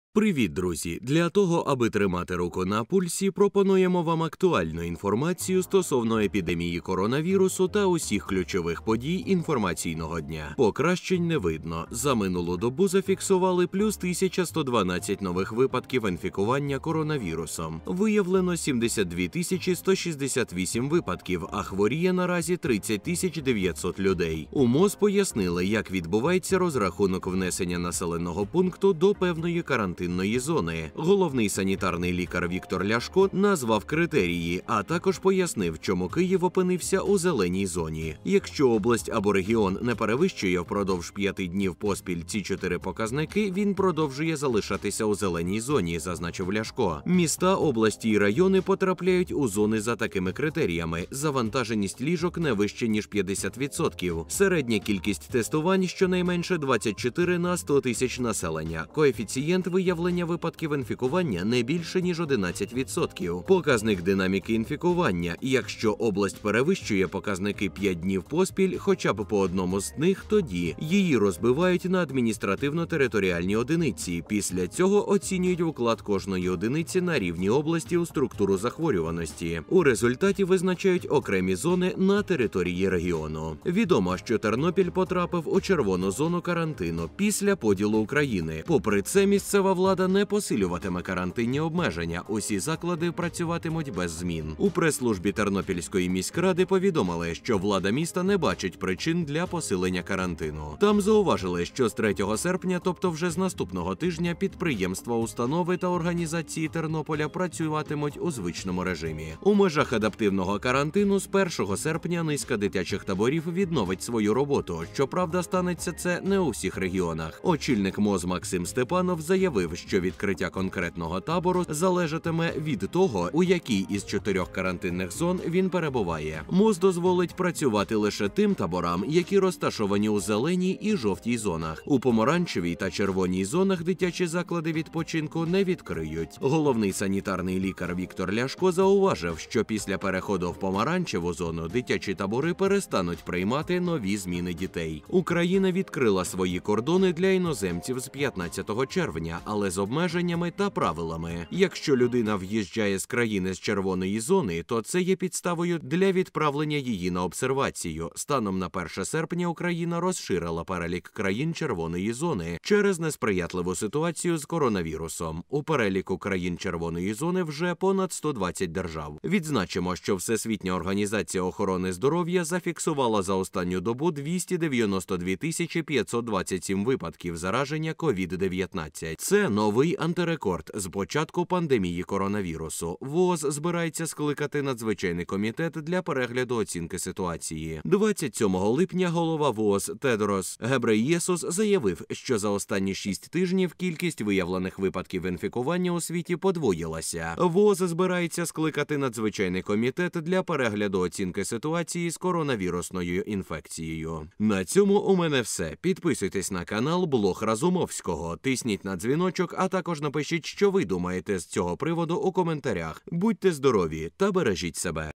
Покращень не видно! 02.08.2020 – Офіційні дані України та світу: 1112 нових випадків за минулу добу
Нові подробиці.